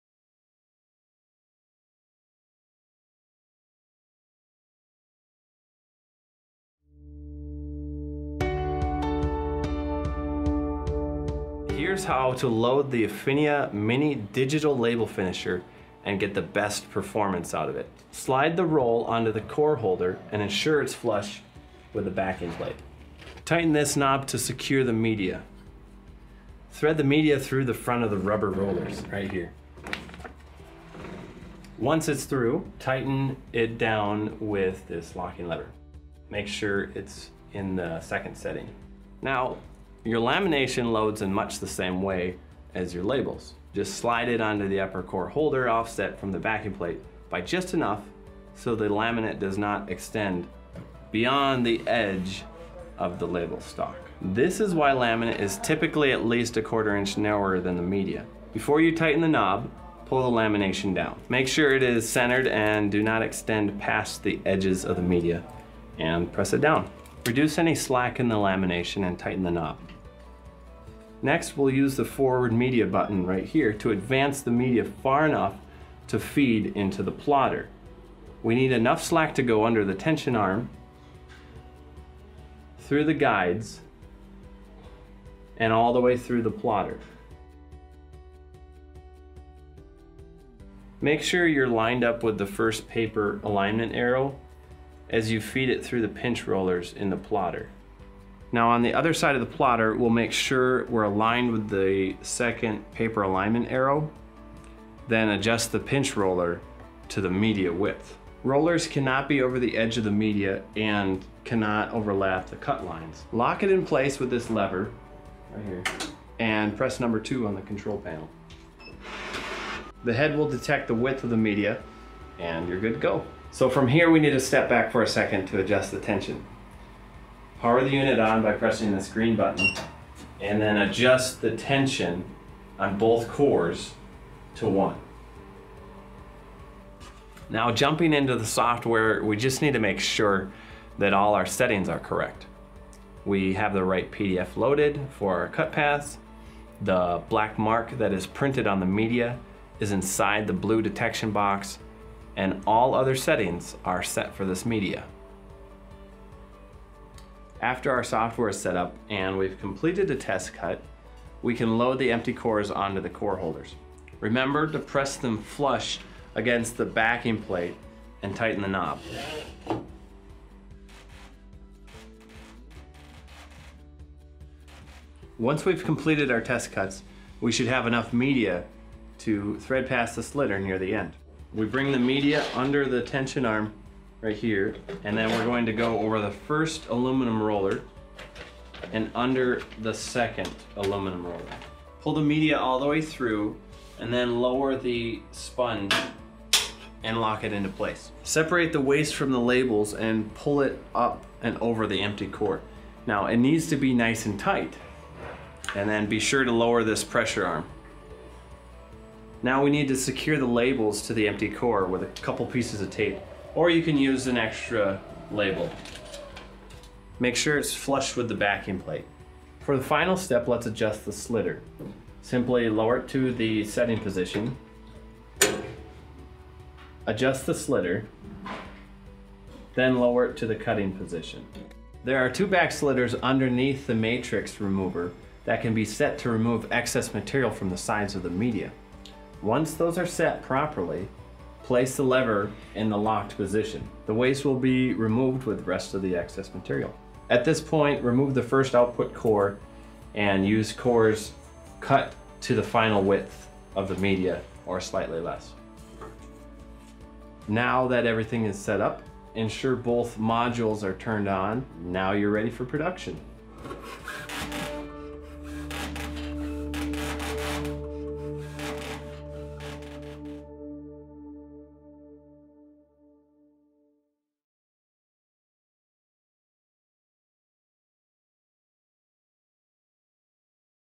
Afinia's DLF140s &DLF220s Digital Label Finisher Basic Setup and Operation
Take a few minutes to learn how simple it is to set up and operate Afinia's Digital Label Finisher in this step by step video.  The DLF 140s and DLF220s Mini series offers a small footprint approach to finishing pre-printed label stock by laminating, cutting unique shapes and sizes, removing waste, slitting down into individual rolls and rewinding. Perfect for any private label or co-packaging operation, and any business that has a large catalog of products that require different label sizes or lamination as added protection.

With flexibility at your fingertips, you can produce unique and production ready labels just in time.

For more DLF mini series support, including access to the owners manual and driver downloads follow this

Afinia Tech Support is available Monday through Friday 8am to 5pm CST.  Their phone number is: